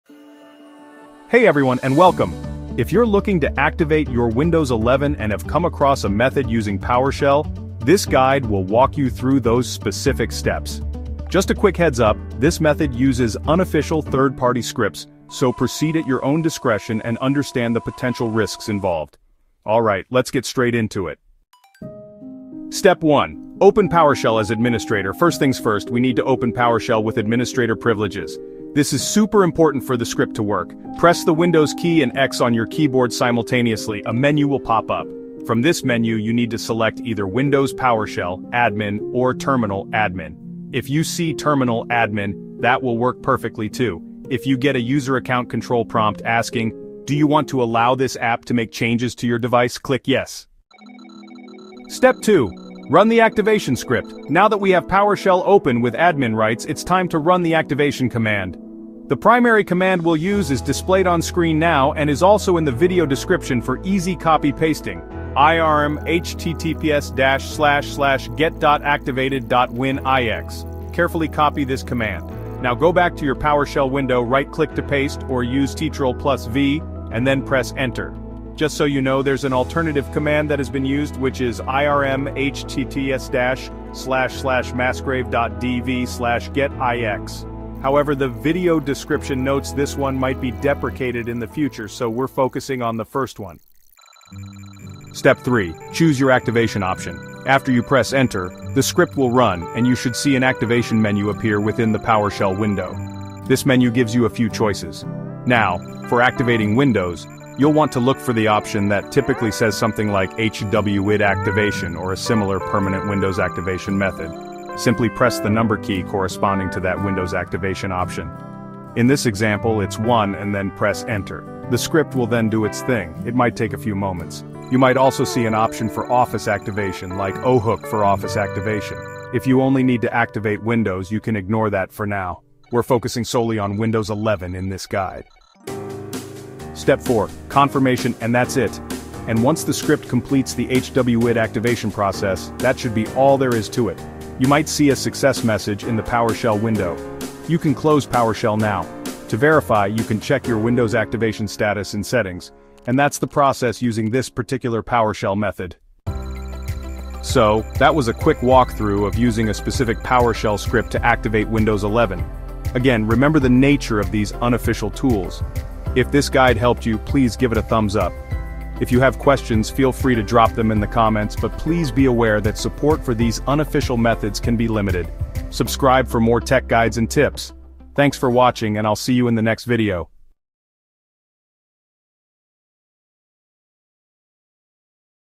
Activate Windows 11 For FREE! 🤫 PowerShell Script Method (2025)
Tired of that "Activate Windows" watermark? In this 2025 guide, I'll show you how to activate Windows 11 for FREE using a specific PowerShell script method. This tutorial walks you through the steps to potentially unlock the full version of Windows 11 and get rid of annoying activation prompts.

Proceed at your own risk and discretion. We are not responsible for any issues that may arise. We always recommend purchasing a genuine Windows 11 license from Microsoft or authorized retailers for legitimate and secure activation.

**In this Windows 11 activation tutorial, you'll learn:**
* How to open PowerShell as an Administrator.
* The exact PowerShell commands needed for this Windows 11 activation method.
* Navigating the script's menu for HWID activation.
* (Potentially) Removing the "Activate Windows" watermark.

**TIMESTAMPED GUIDE:**
0:00 - Intro & Important Disclaimer
0:45 - Step 1: Opening PowerShell as Administrator
1:30 - Step 2: Finding and Copying the PowerShell Command
2:15 - Step 3: Running the Activation Script in PowerShell
3:00 - Step 4: Choosing the Correct Activation Option (HWID)
3:45 - Checking Activation Status & Outro

This guide is for users looking for information on alternative Windows 11 activation solutions and PowerShell scripts like `irm | iex` or the `massgrave.dev` utility for Windows. If you're searching for a free Windows 11 product key or license, this video explores one such community-driven method.

💬 Have questions or used this method? Share your experience in the COMMENTS below (but please remember the disclaimer).

**Note:** The commands shown are subject to change by their developers. This video reflects the method as of May 2025.